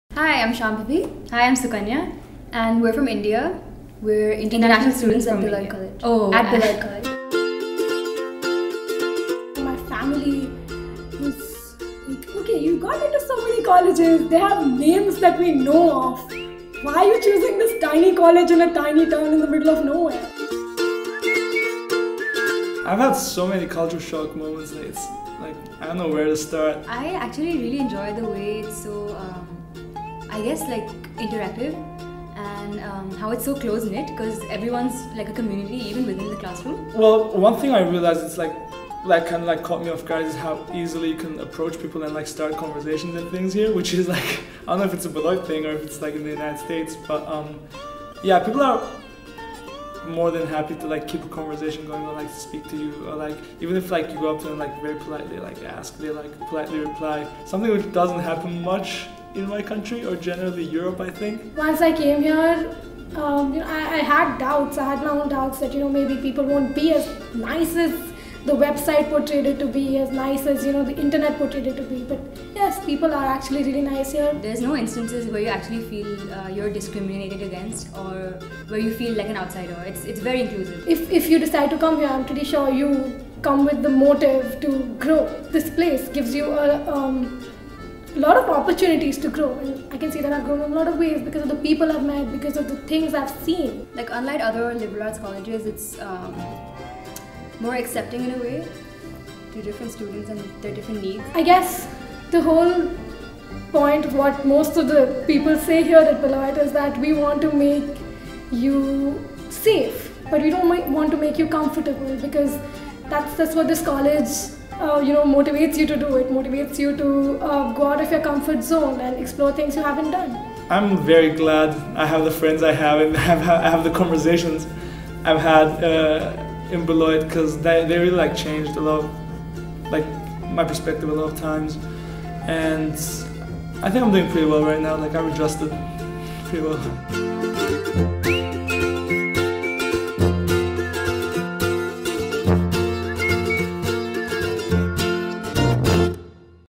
International Students @ Beloit College
Our students come from all over the world, but at Beloit College, we’re a close-knit community. Listen as international students Shambhavi Upadhyaya'19, Sukanya Upadhyaya'19, Varak Mouradian'19, and Yashodhara Kundra'20 describe their experiences.